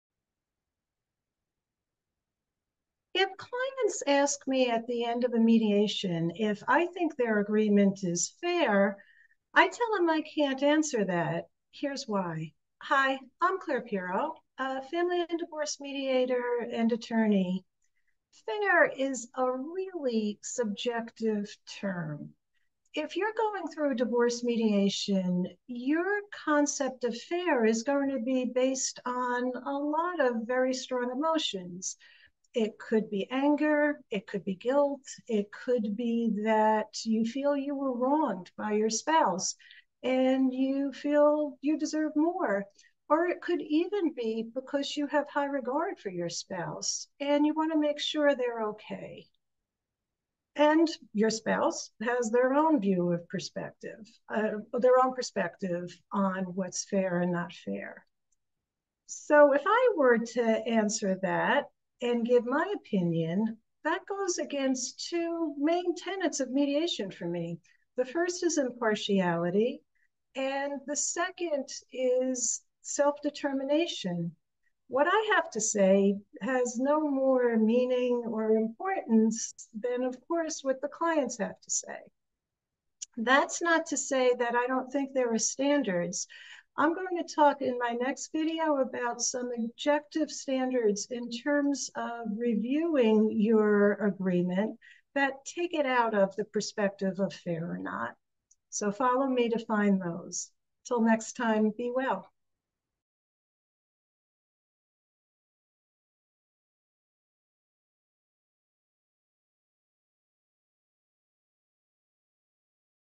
The Importance of Subjectivity and Impartiality
In this video, Clare discusses why, even when asked, she refrains from providing a subjective judgment on whether a mediation agreement is fair. She emphasizes that the concept of fairness is highly subjective, and underscores the importance of impartiality and self-determination in mediation, stating that her opinion should not hold more weight than the clients' perspectives.

If you would like more information about mediation, prenups, and family law, check out my